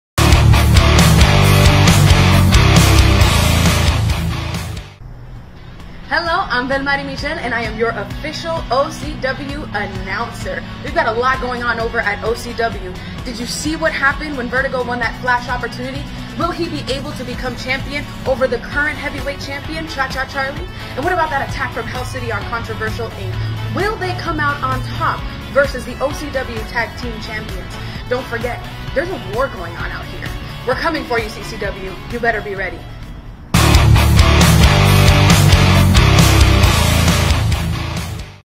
OCW's Official Announcer Velmary Michelle has the latest news and scoops
Original Championship Wrestling official Announcer Velmary Michelle is keeping you up to date on the latest OCW News.